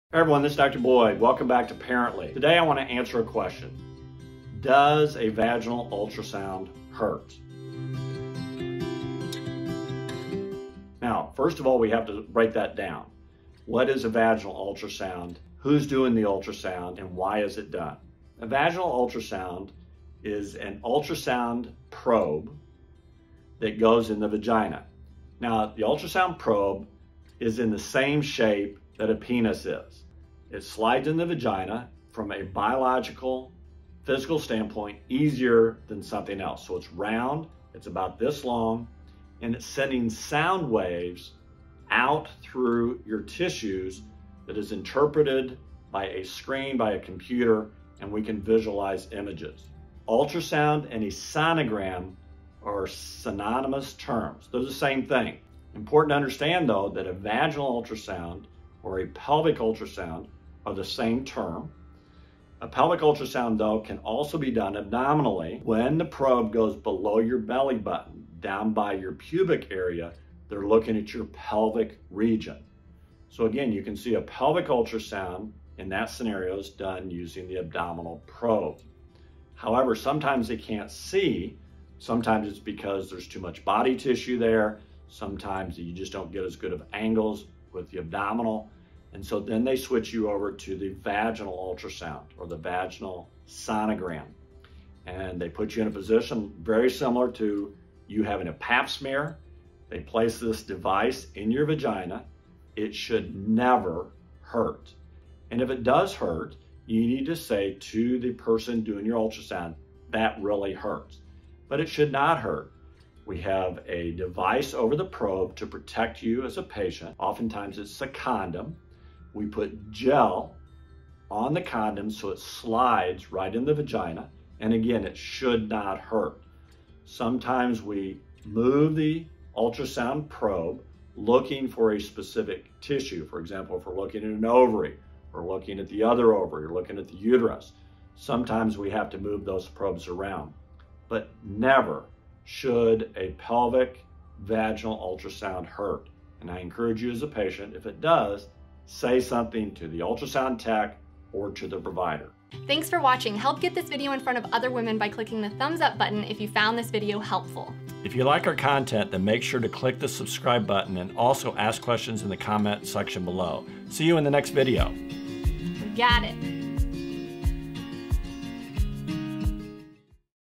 Does a vaginal ultrasound hurt
Vaginal ultrasound aka vaginal sonogram (sono) should not hurt! Please talk to your healthcare provider if you experience pain.